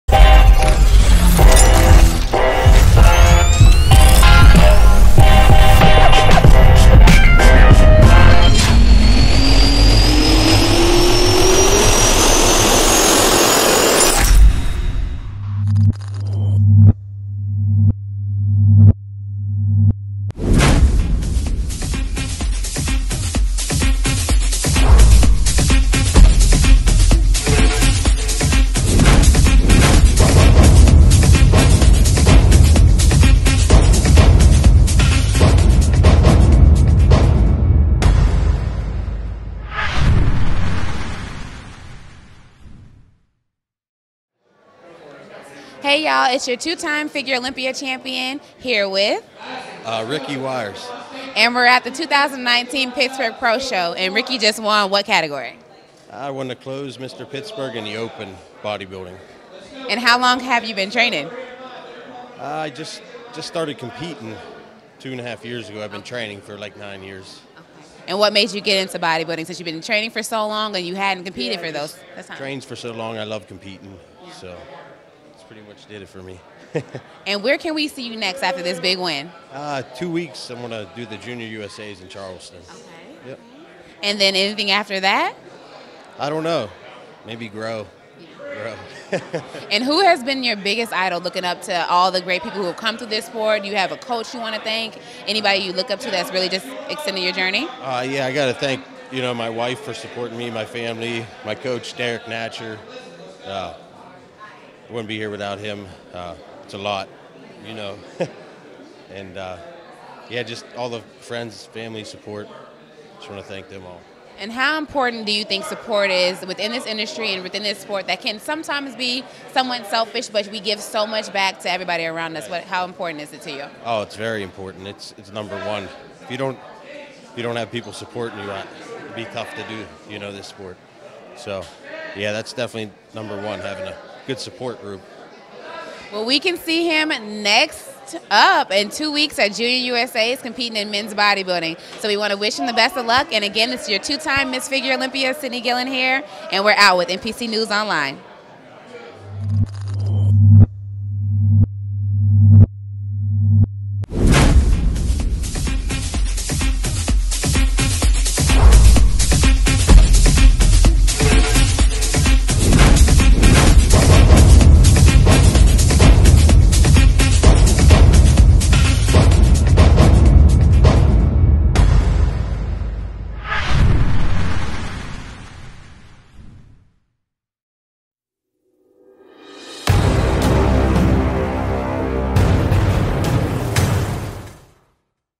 2019 NPC Pittsburgh Championships Bodybuilding Overall Winner Ricky Weiers
2019 NPC Pittsburgh Championships Bodybuilding Overall Winner Ricky Weiers After Show Interview With 2x Figure Olympia Champion Cydney Gillon